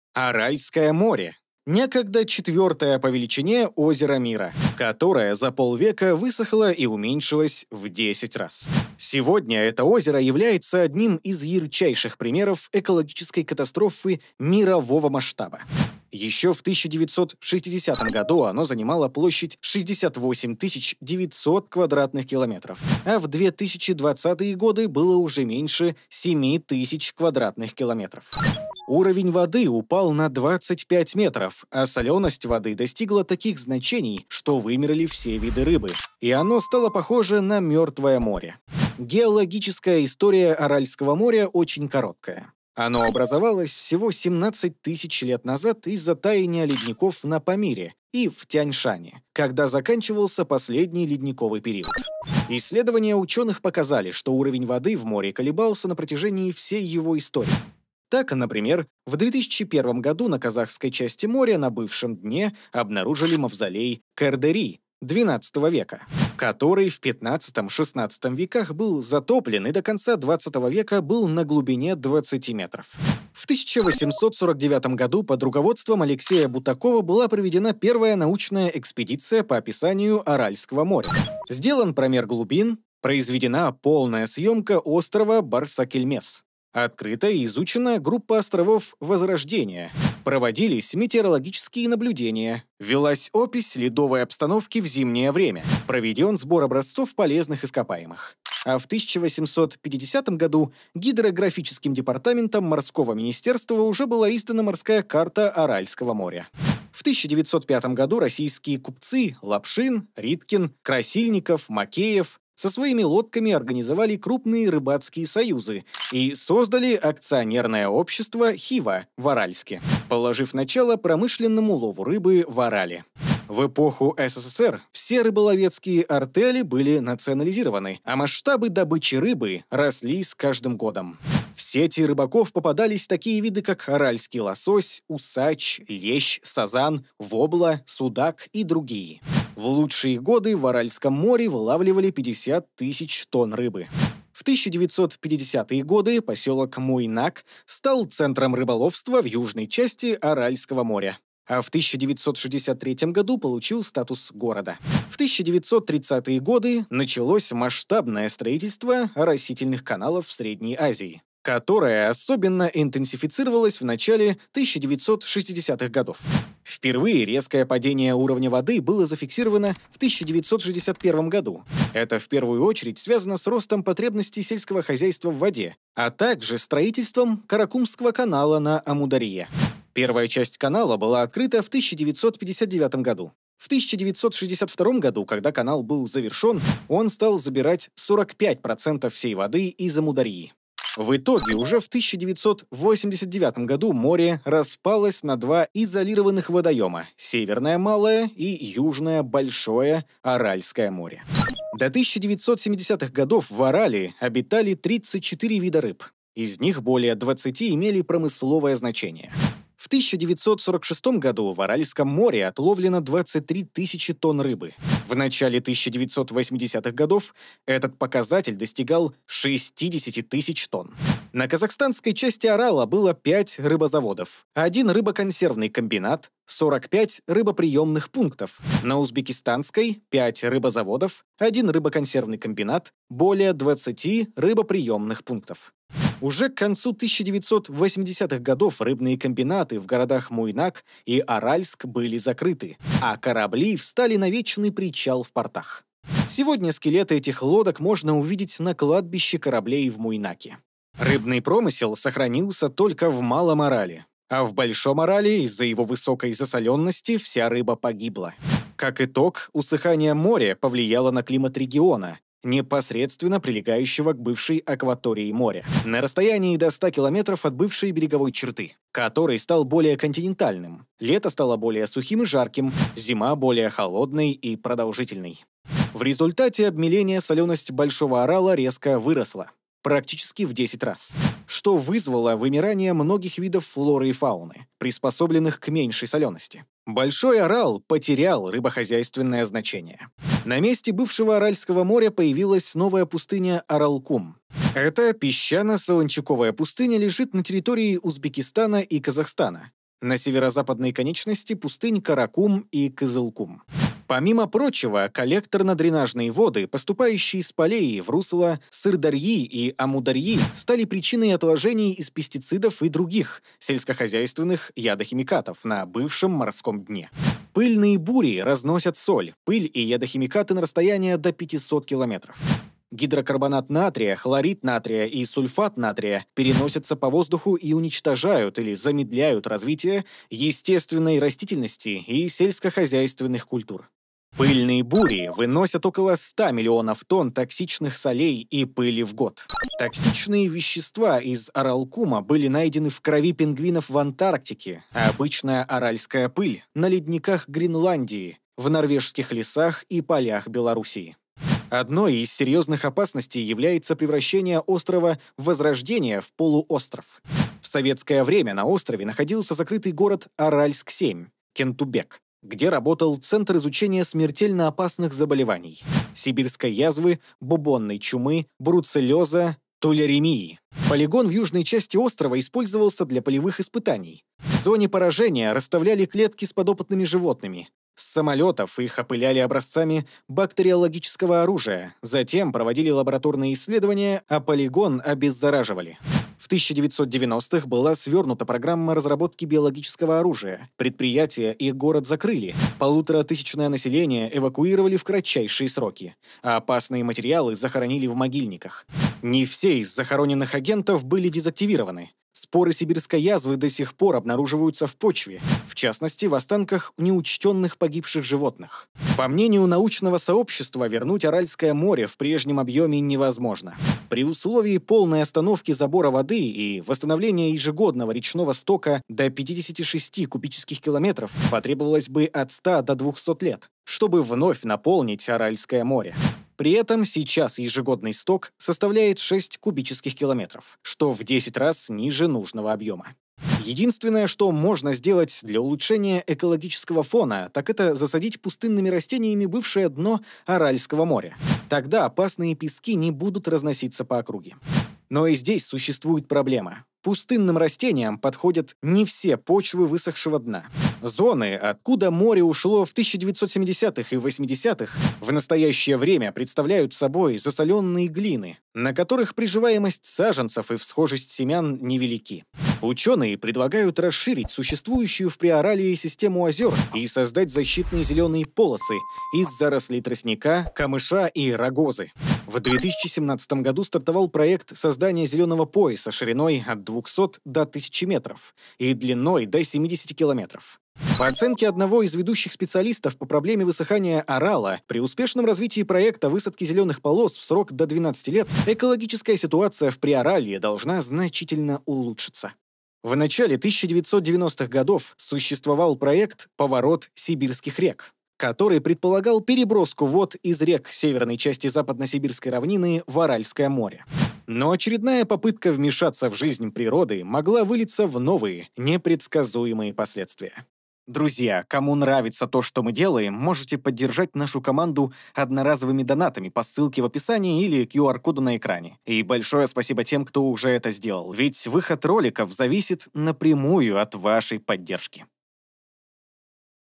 ☣️Это МОРЕ превратилось в ОПАСНУЮ ПУСТЫНЮ. Катастрофа Аральского Моря
Аральское море за 50 лет уменьшилось в 10 раз. Сегодня - экологическая катастрофа аральского моря - это бедствие мирового масштаба. Главные  причины высыхания аральского моря - это человеческий фактор. В этом ролике мы разберёмся почему высохло аральское море, а также выясним каково состояние бывшего моря сейчас.